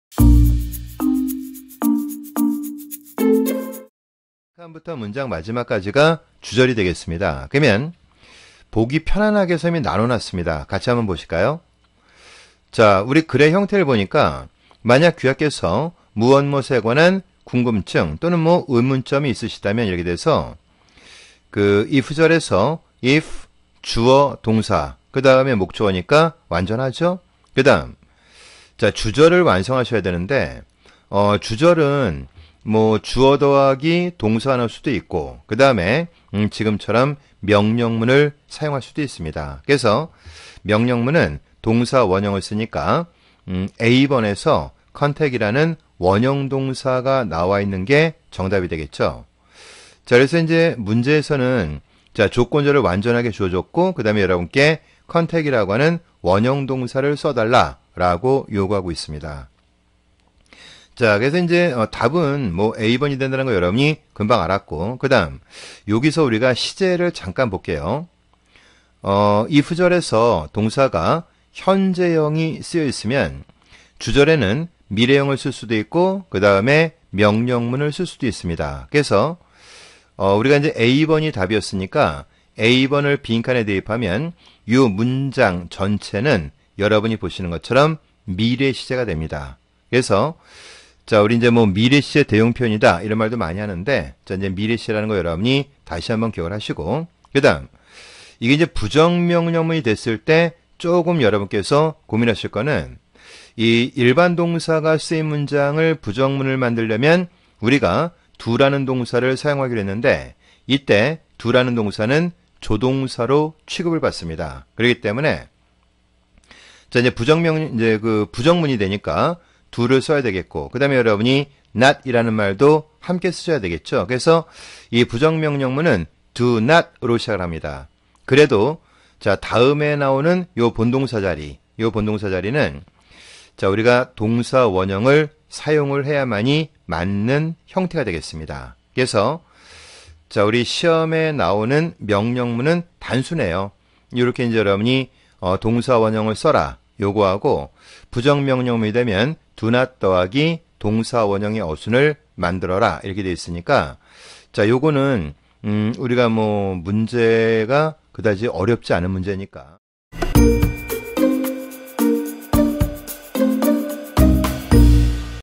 [안쌤토익] 명령문 문제 유형풀기
토익 파트5 명령문 문제 유형,
쉽게 푸는 방법을 배워봅시다.